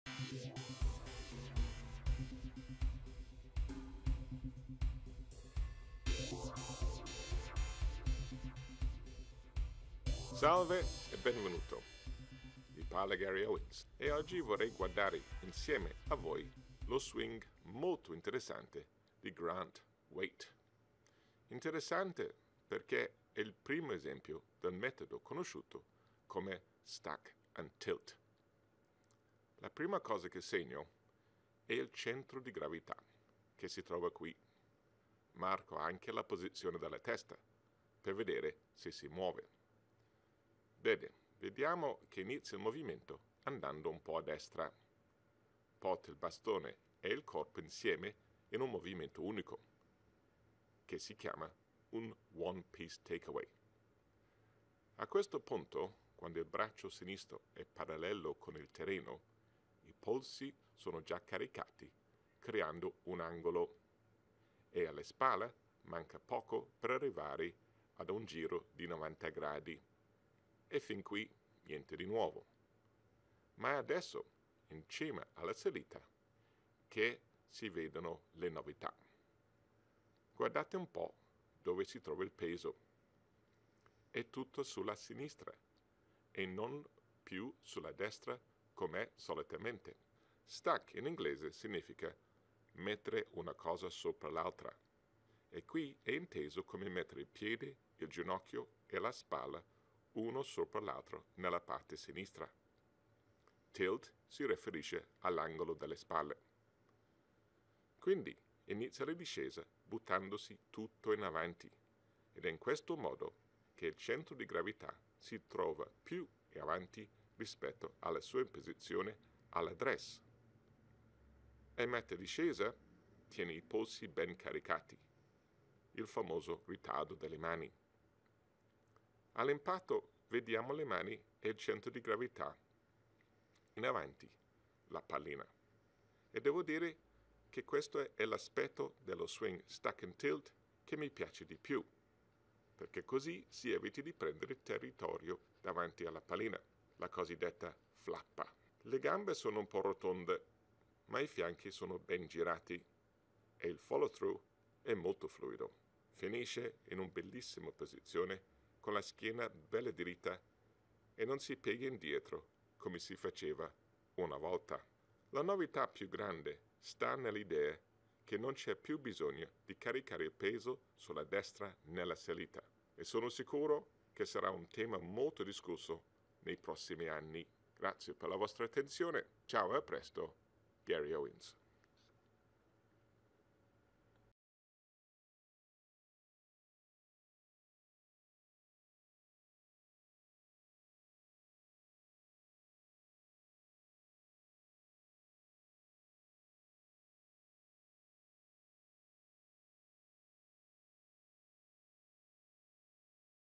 Chi è Grant Waite?
Un'analisi dello swing di Grant Waite, primo esempio del "Stack and Tilt".